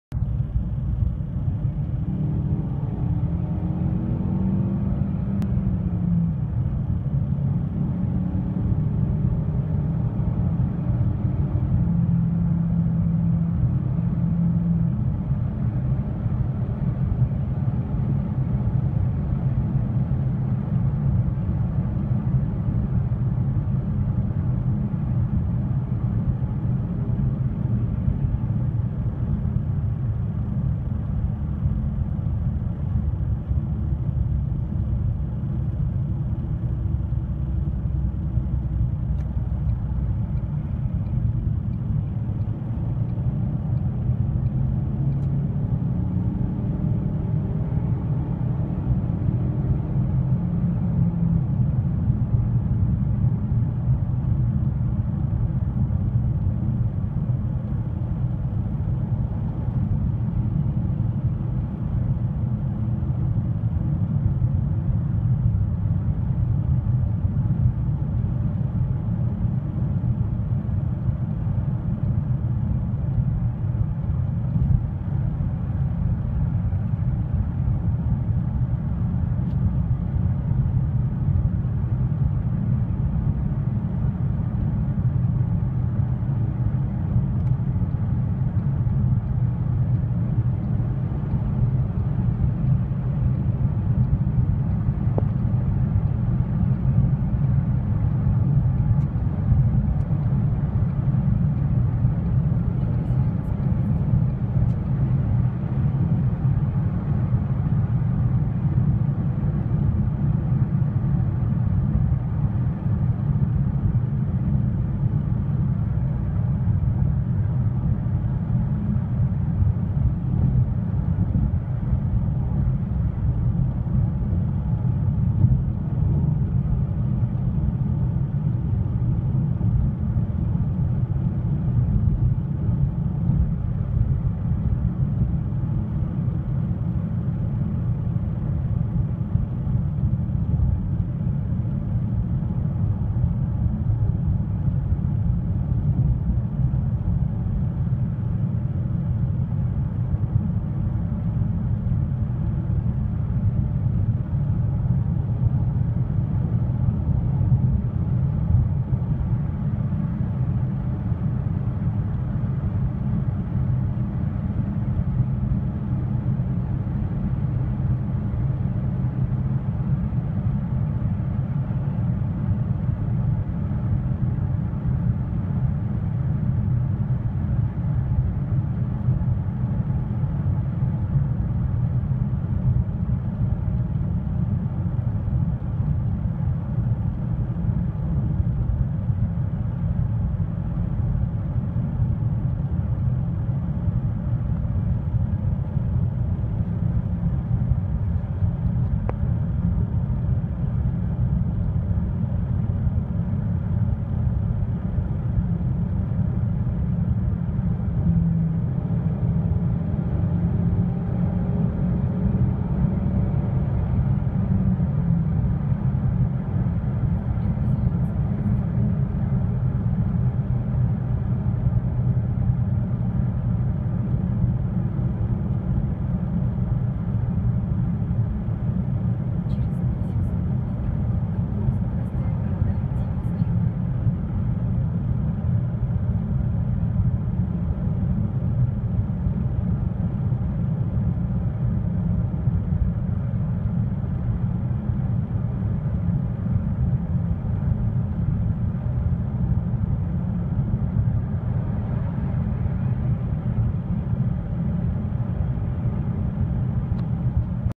Веста АМТ
снял на видео работу робота АМТ, до адаптации.